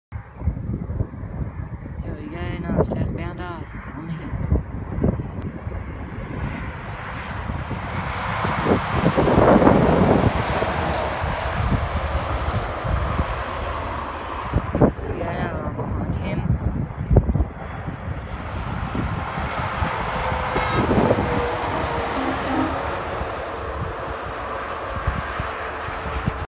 Canny truck on the Hume Highway
This was filmed on the 15/11/19 heading south bound past Lade Vale NSW with a good horn show